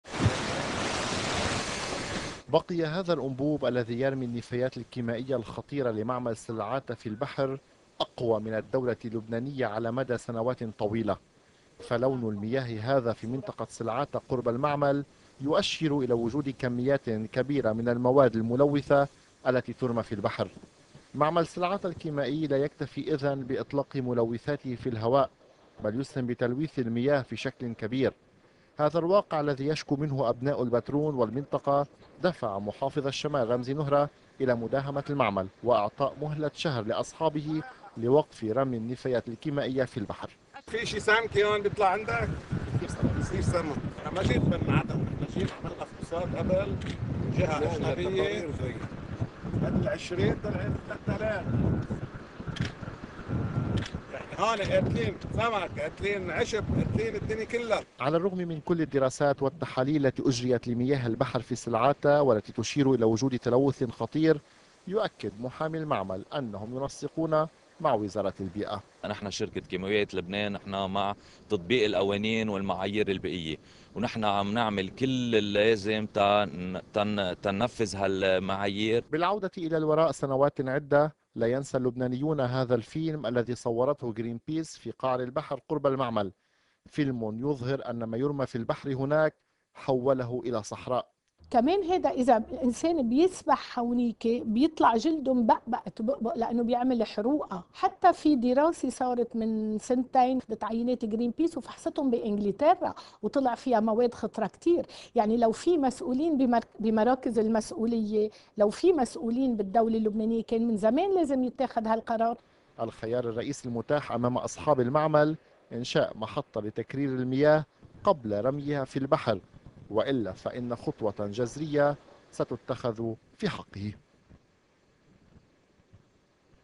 LBCI News - إنذار معمل "كيماويات سلعاتا"
دهم محافظ الشمال القاضي رمزي نهرا "شركة كيماويات سلعاتا" في البترون وأعطاها مهلة شهر لوقف عملية رمي النفايات في مياه البحر.